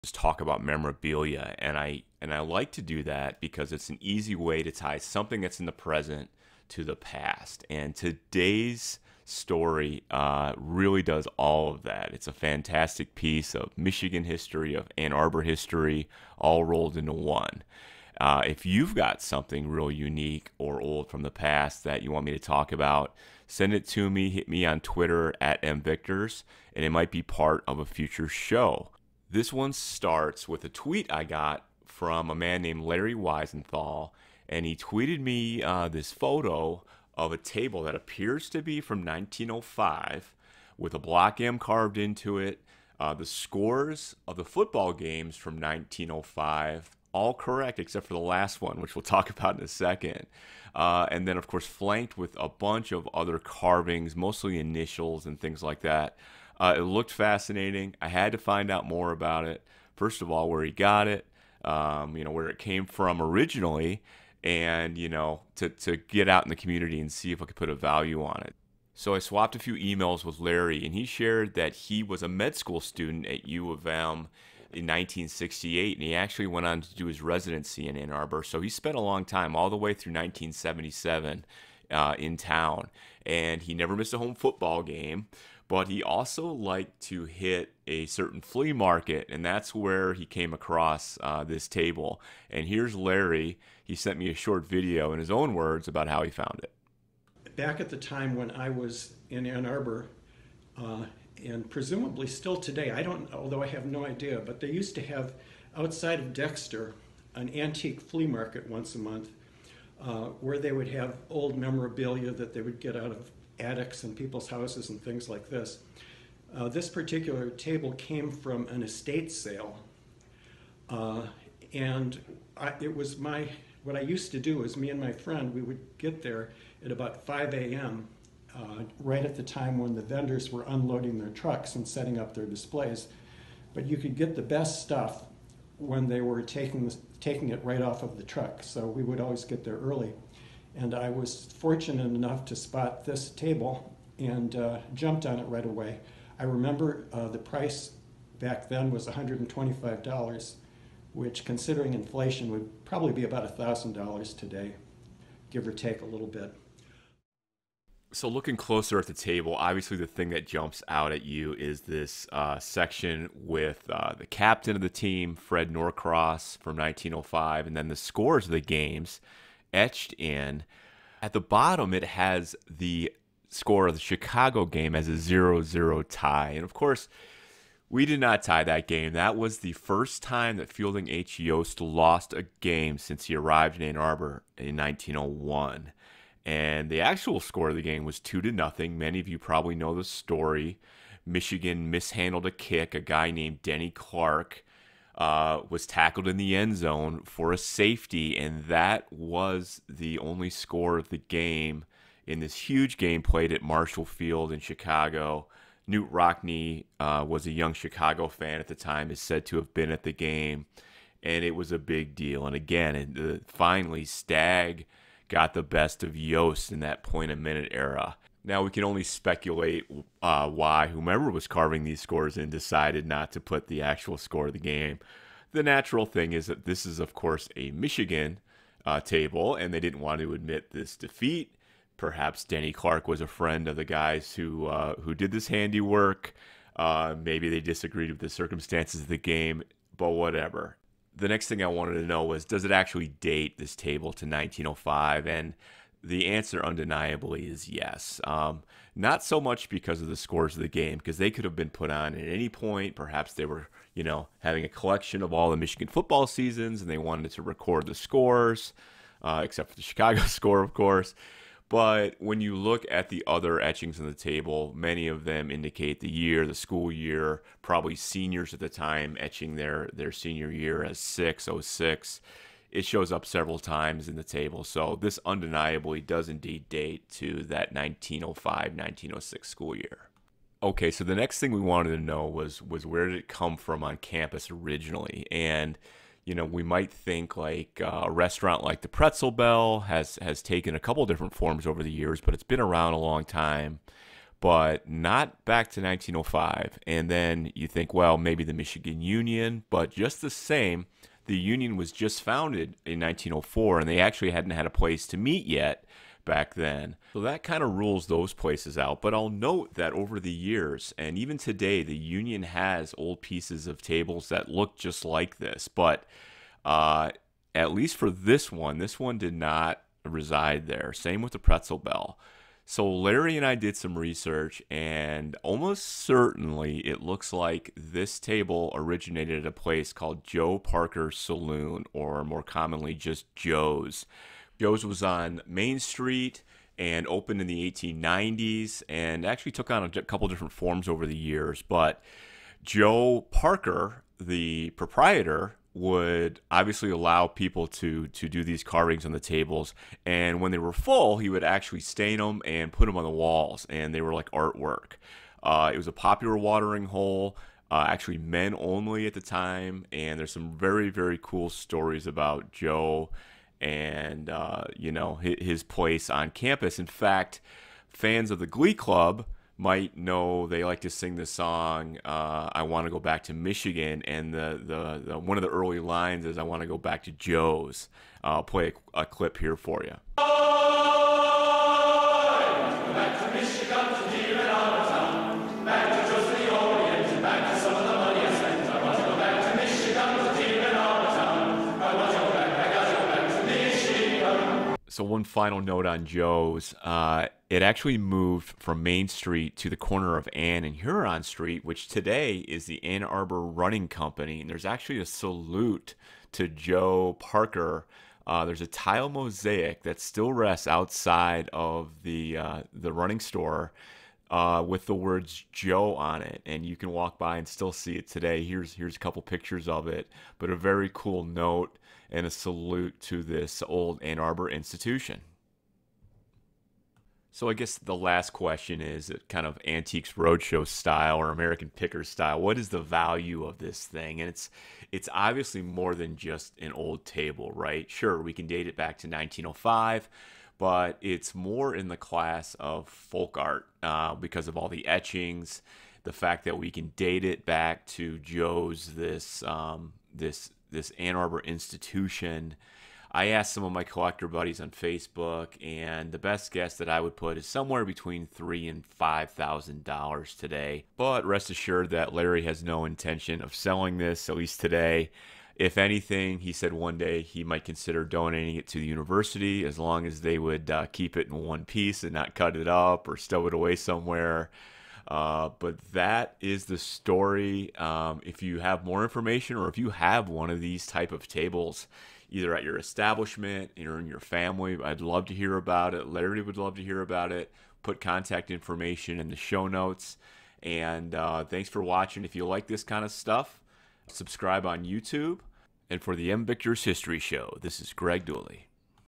Going Back to Joe's (1905) | MVictors History Show
This episode explores a 1905-06 table bought at a Dexter, Michigan flea market.  It features the scores from the 1905 Michigan football season and many carved initials, presumably from U-M students, from that year.  The table almost certainly originally came from Joe Parker's saloon on Main Street Ann Arbor from that year.  For more: